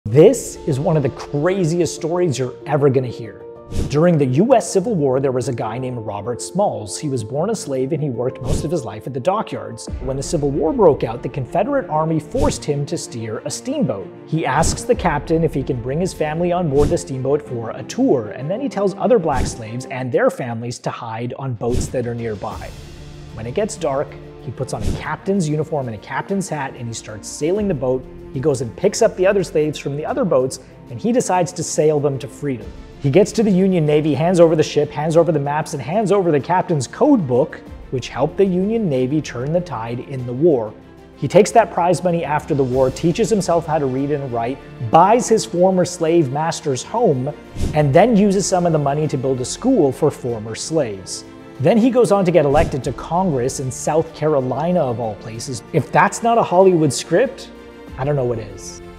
How Robert Smalls Seized a Confederate Ship to Freedom
@Goodable is the #1 place in the world to watch good news.

Made possible by the Ontario Creates Fund.